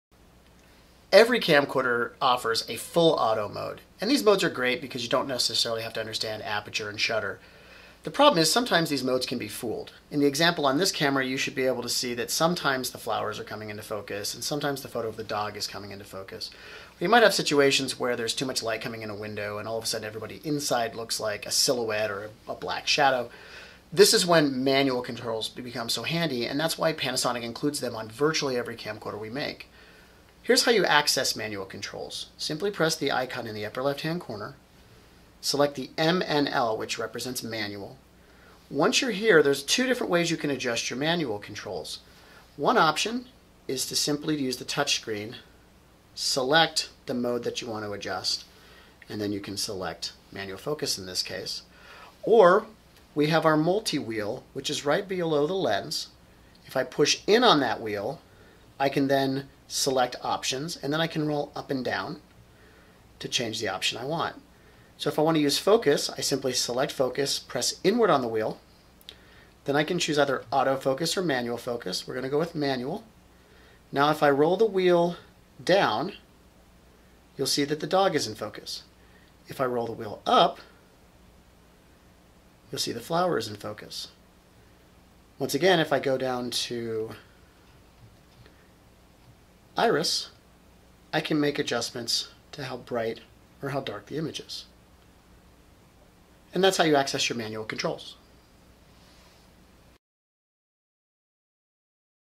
Panasonic - Camcorders - Function - How to Use Manual Controls
This is a quick tutorial on how to take advantage of the manual controls on most Panasonic Camcorders.

The camera shown in this video is the HC-WX970, screen shots vary by camera.